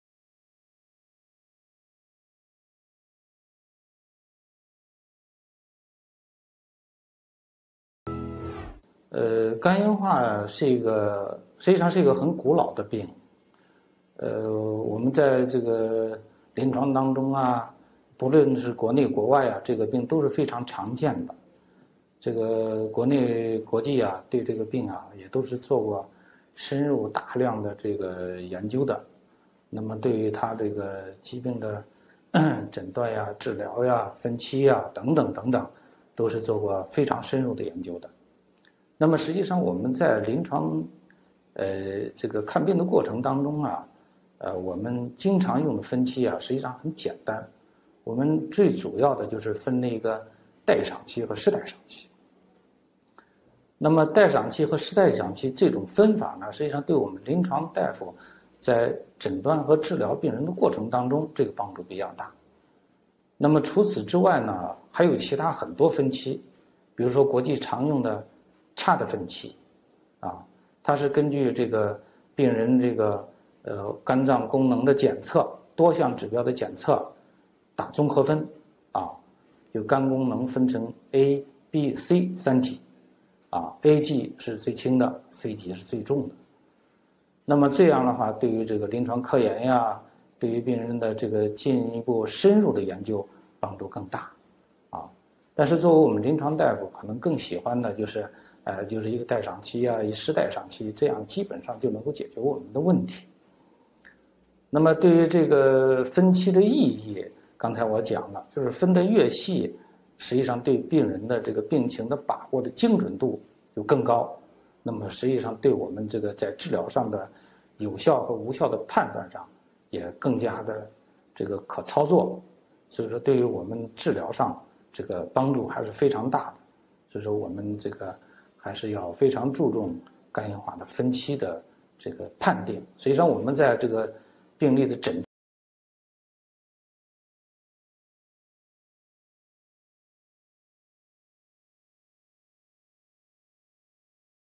肝硬化是怎么分期的 尚建中 四川华西肝病研究所
肝硬化是实际上是一个很古老的病，经常用的分期实际上是很简单的，我们最主要的就是分代偿期和失代偿期。除此之外，还有其他很多分期。但是作为我们临床大夫，可能更喜欢的就是就是一个代偿期，失代偿期期这样基本上就能够解决我们的问题。

How does cirrhosis stage
Cirrhosis is actually a very old disease, The commonly used stages are actually very simple, We are mainly divided into compensation period and loss of compensation period. besides, There are many other stages. But as our clinical doctors, Maybe what I like better is a compensation period, In this way, we can basically solve our problems.